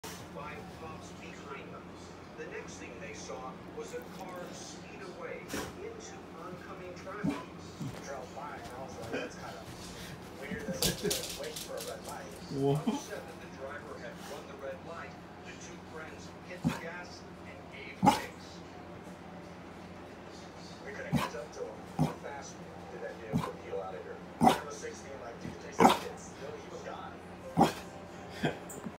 American bully needs to poop!
Misty, a big baby, telling me she’s gotta go! Lol